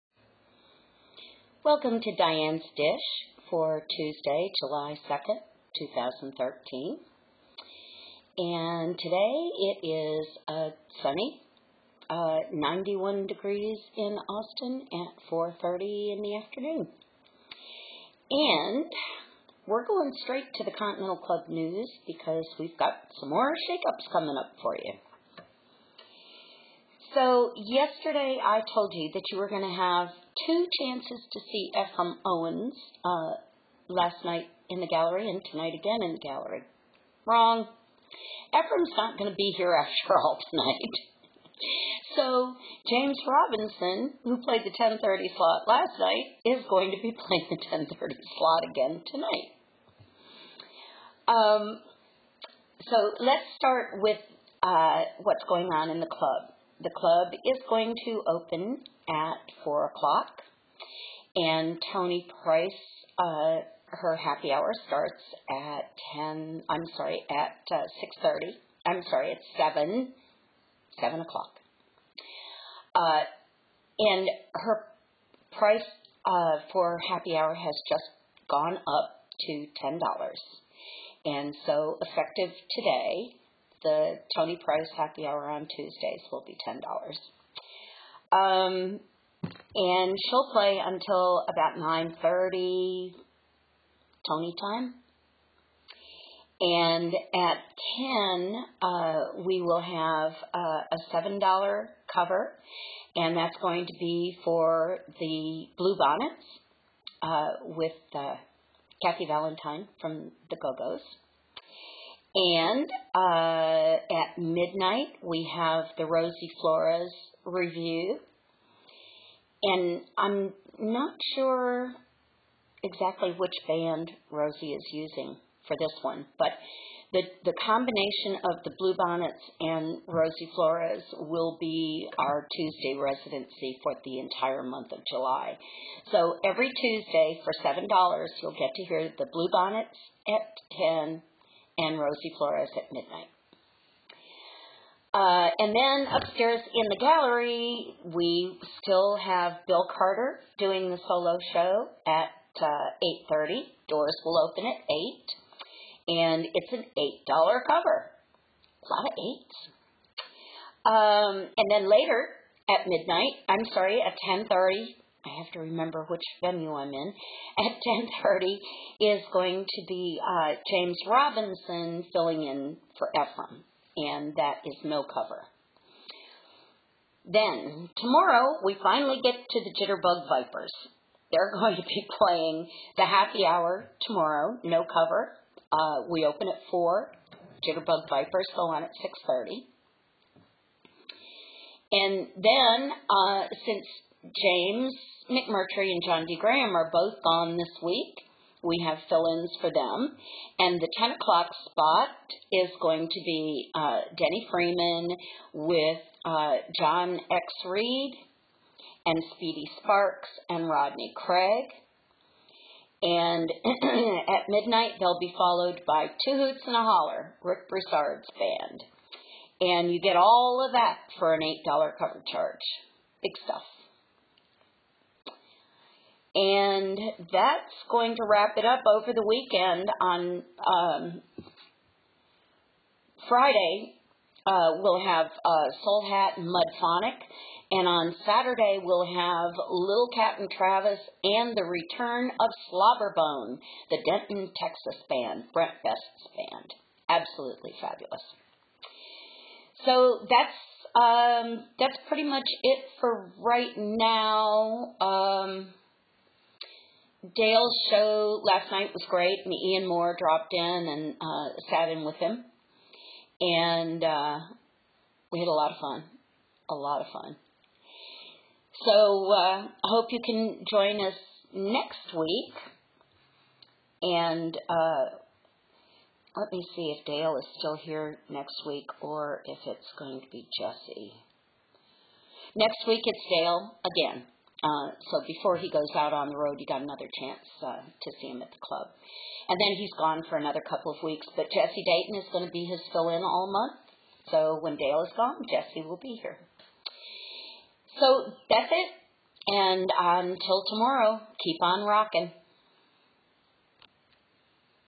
Dianne's Daily Dish for Tuesday 7/2/13 - Vol. 1 #20 by Dianne Scott
This is a daily Vlog about the happenings at The Continental Club (1315 S. Congress Ave), and The Continental Club Gallery (1313-A S. Congress Ave) and around Austin TX.
78704 - it's not just a zip code; it's a way of life.
We have no ticketed shows at this time.
Streamed by Dianne Scott in Austin, TX.